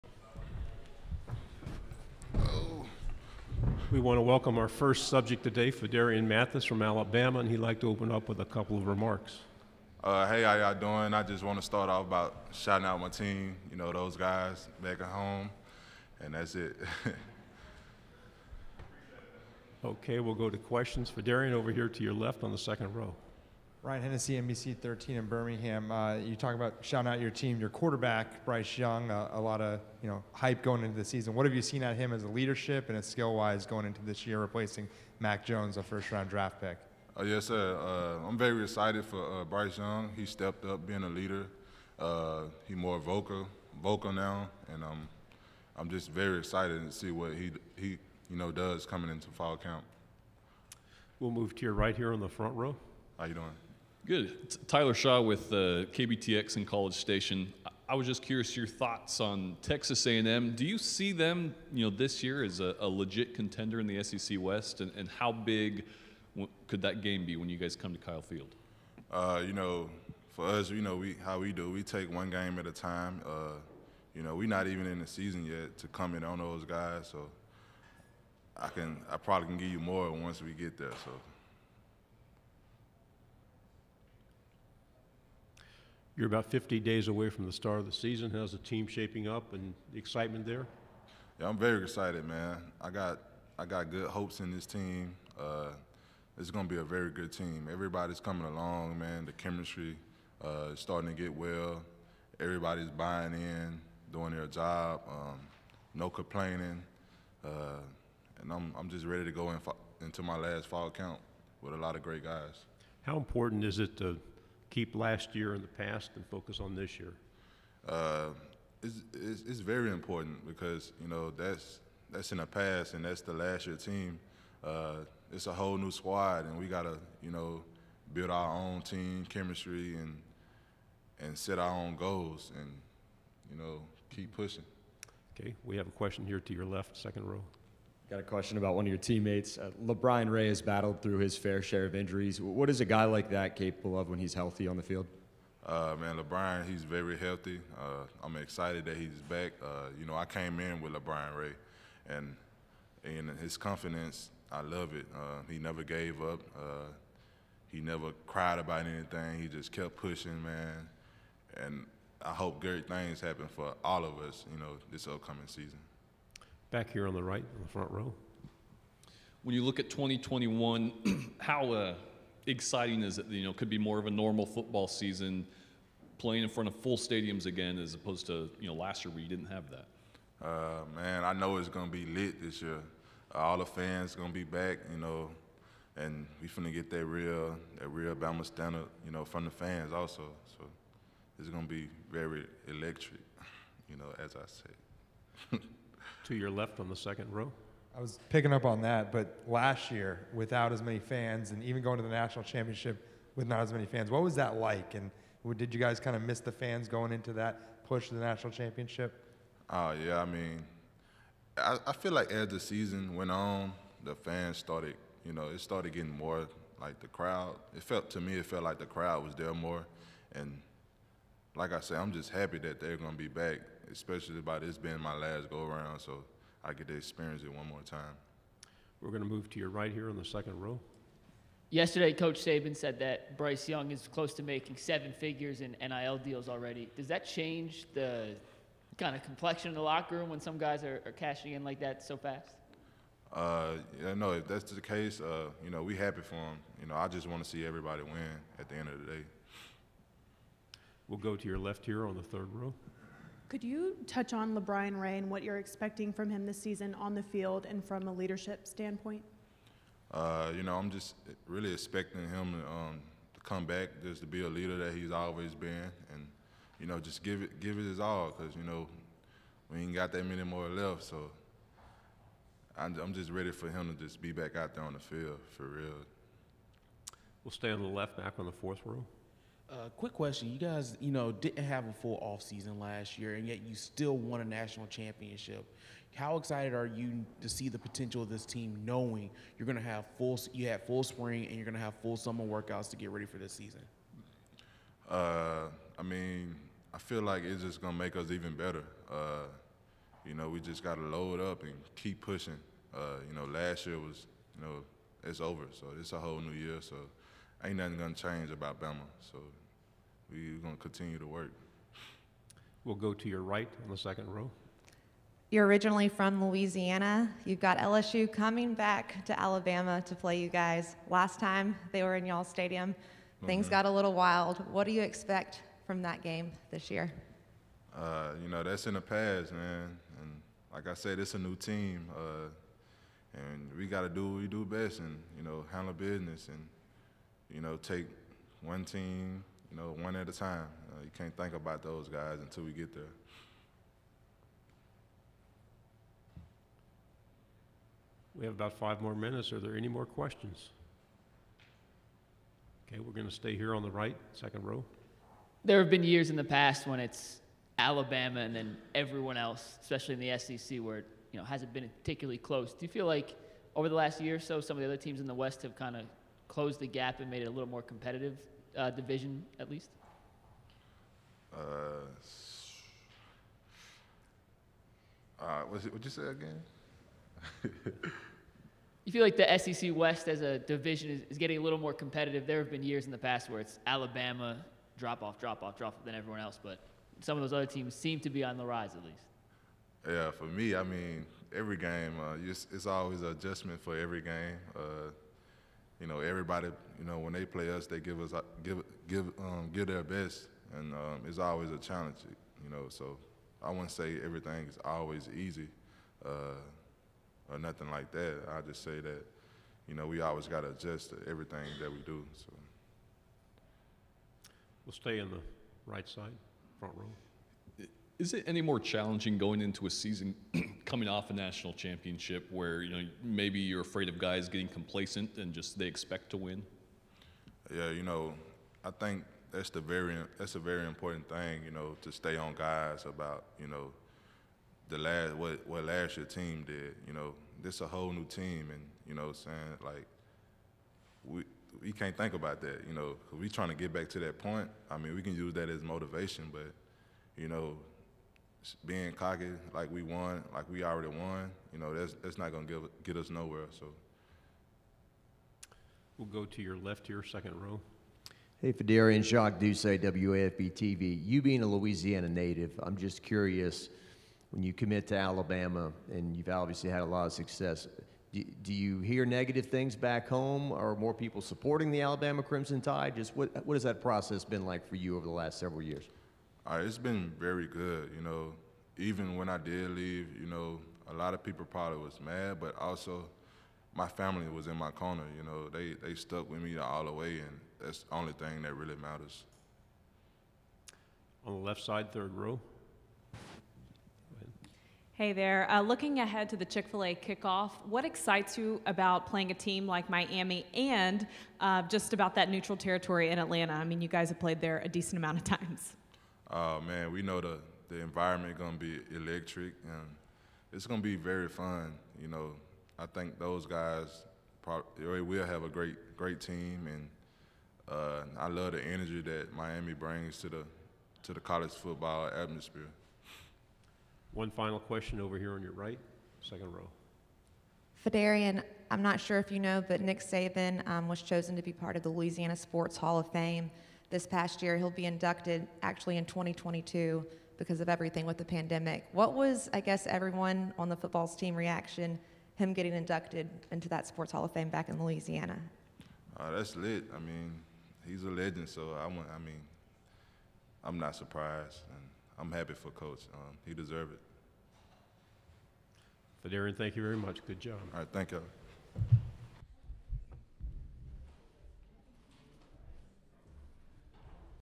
Phidarian Mathis SEC Media Days 2021 Alabama Crimson Tide defensive lineman (small room) July 21
Phidarian Mathis video from Hoover, Alabama SEC Media Days 2021